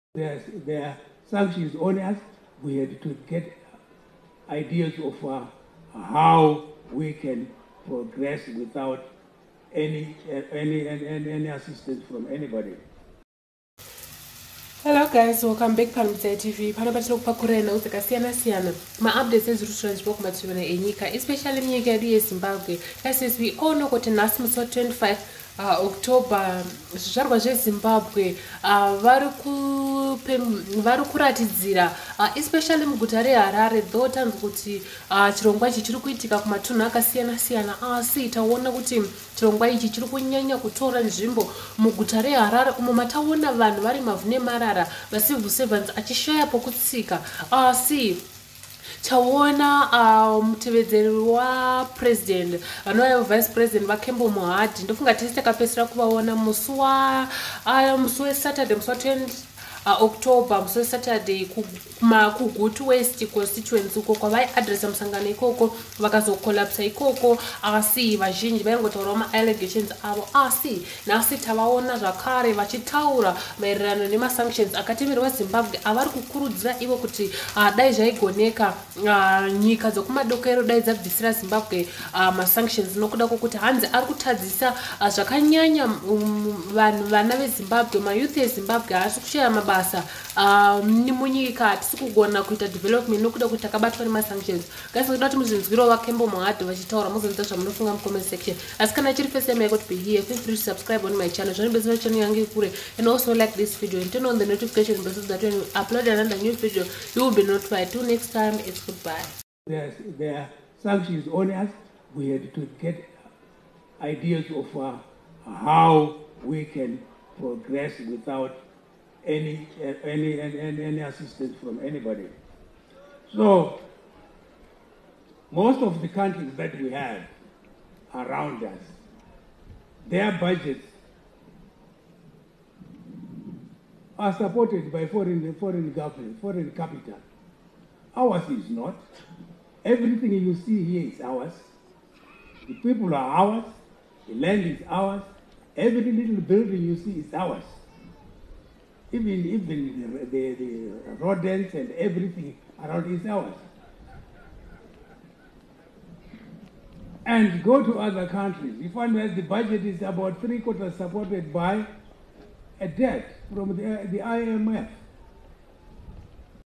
🤯🫢Inzwai VicePresVaKembo Mohad vati masanctions anofanira kubviswa nokut ndiwo atiuraira nyika🙊
😳😳Inzwai Vice Pres VaKembo Mohad vat ma sanctions anofanira kubviswa nokut ndiwo atiurayira nyika🙉